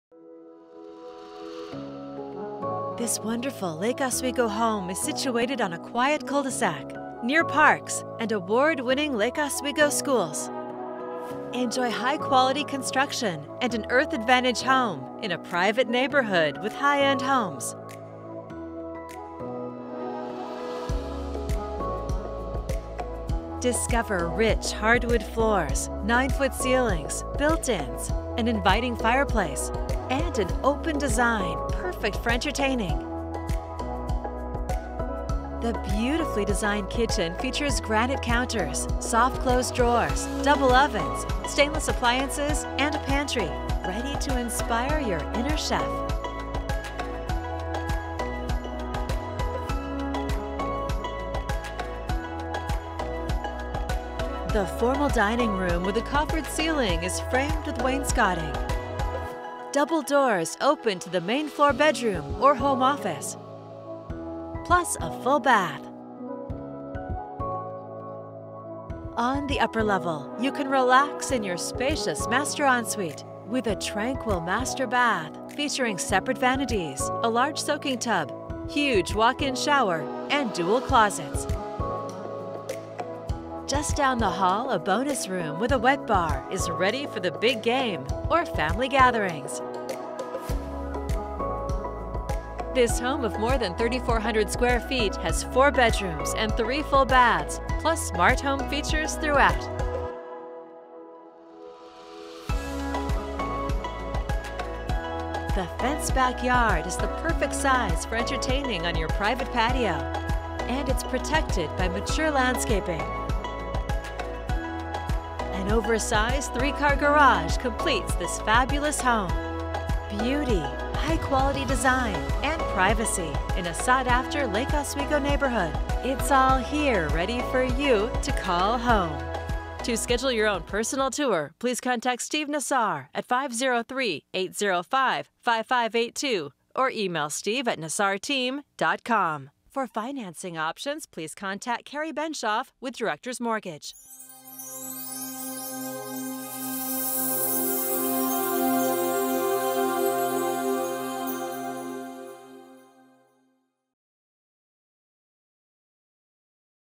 Exceptional Lake Oswego Home ~ Video of 5598 Luce Ln. ~ Lake Oswego homes
Fantastic Lake Oswego home rests in a quiet cul de sac neighborhood. Features an open concept floor plan with smart home features throughout! 3432 sqft | 4 bedrooms | 3 baths. Contact Steve Nassar with Premiere Property Group.